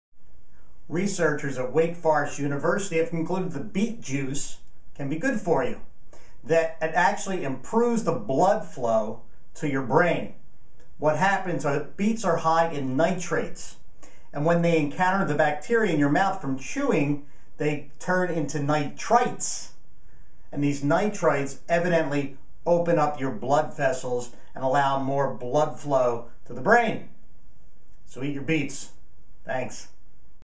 Beets Good for YOU!
Researchers at Wake Forest University's Translational Science Center have found that beet juice can improve blood flow to the brain in older adults.

MRI's of 14 older adults who consumed a high-nitrate diet for four days showed that blood flow to the white matter of the frontal lobes increased in these patients. This area of the brain is linked to the kind of degeneration associated with dementia and cognitive problems, Medical News Today reported.
Posted by Anthony Cirillo at 9:00 AM 0 comments Links to this post